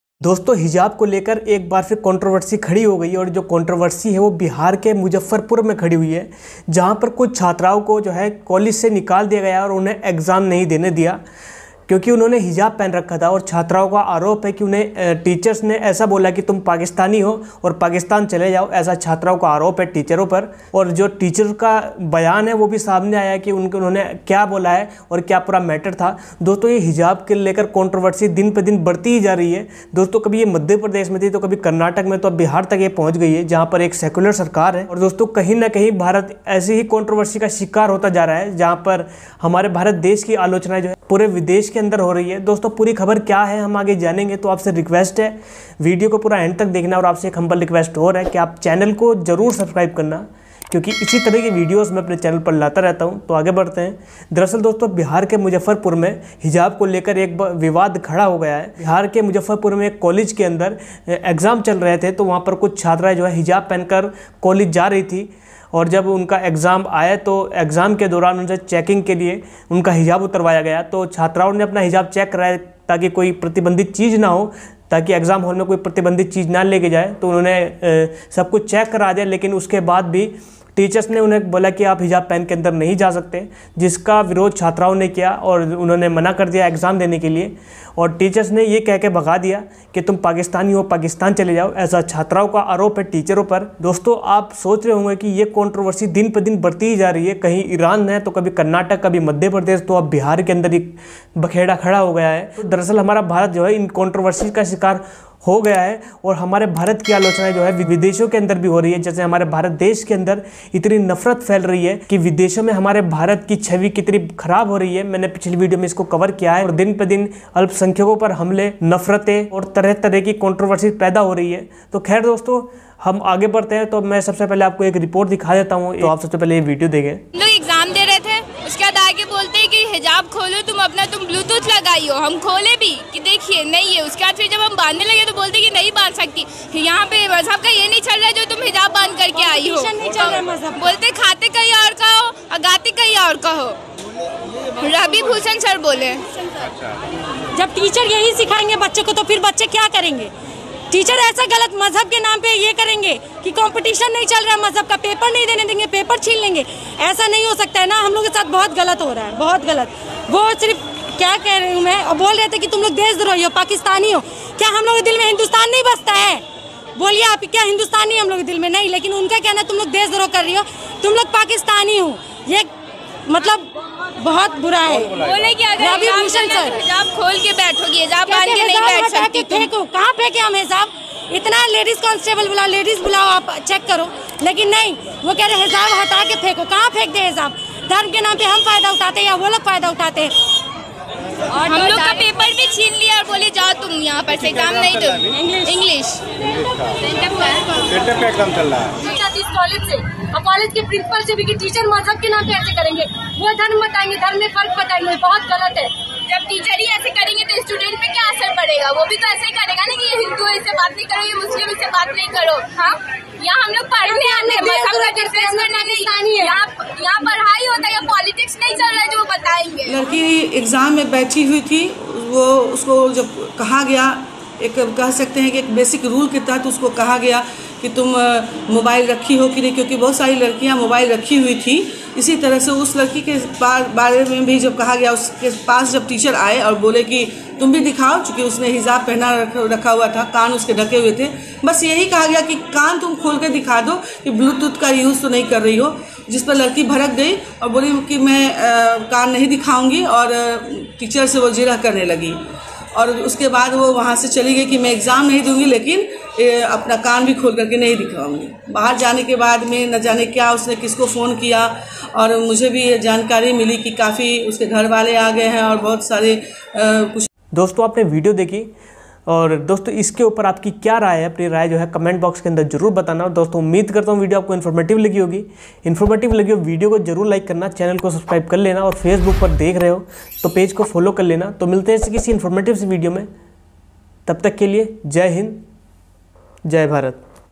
Bihar Hijab Controversy: मुज्जफरपुर के कॉलेज में जबरन hijab हटाने का आरोप, देशद्रोही बता बाहर निकाला
MuzaffarPur के MDDM womans College Exam में Hijab पर Controversy | Bihar News | Latest News in Hindi

एक तरफ हिजाब को लेकर अंतरराष्ट्रीय स्तर पर बहस छिड़ी हुई है, वहीं अब बिहार के मुजफ्फरपुर में भी हिजाब को लेकर एक नया बवाल शुरू हो गया है. शहर के प्रतिष्ठित महिला कॉलेज MDDM कॉलेज की मुस्लिम छात्राओं ने हिजाब को लेकर कॉलेज प्रशासन व प्रोफेसर पर बड़ा आरोप लगाया है. छात्राओं का कहना है कि शिक्षक ने उन्हें हिजाब हटाकर आने को कहा और जब उन्होंने इसका विरोध किया तो उन्हें देशद्रोही बताते हुए पाकिस्तान चले जाने को कहा गया